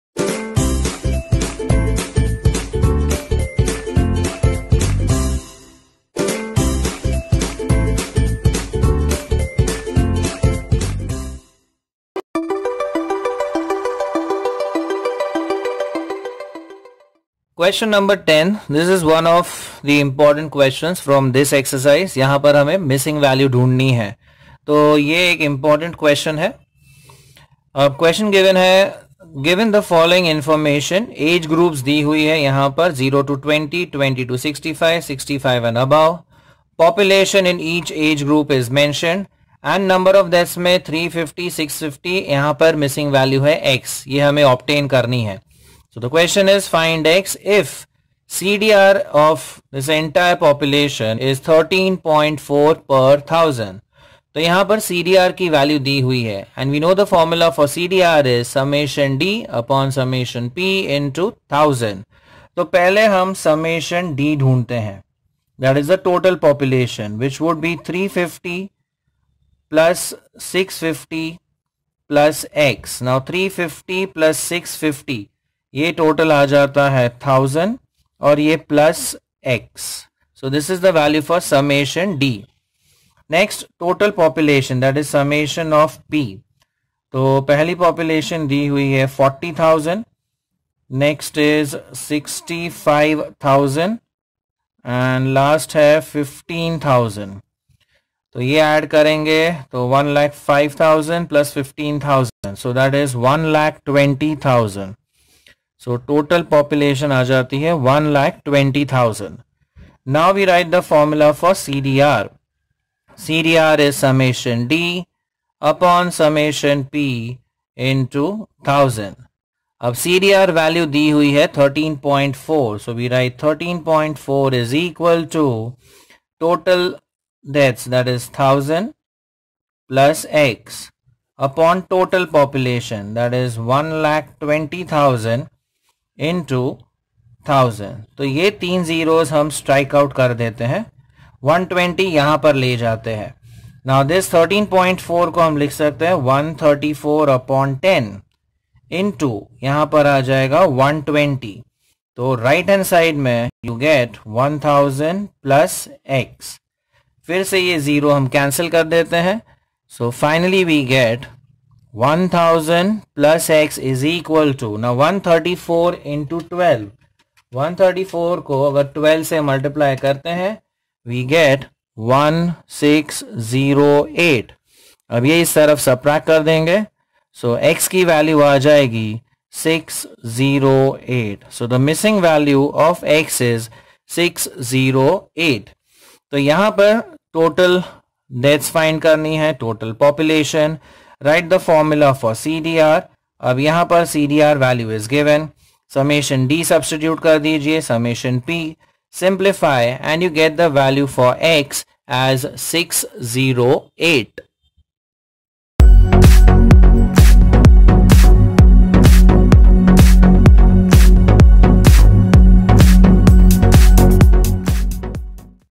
Demography - HSC - SYJC - 12th COMMERCE - Chapter 4 - Maths 2
Solution to Ex 4.1 Qs.10